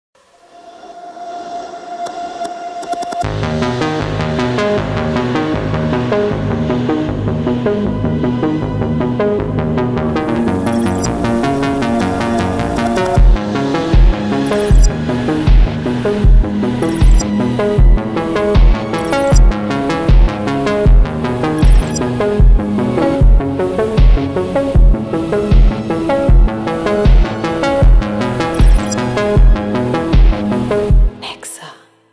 Inspiring elegance
The Ciaz turns heads with its elegant exterior. From the front fascia all the way to the rear, it’s exquisitely designed.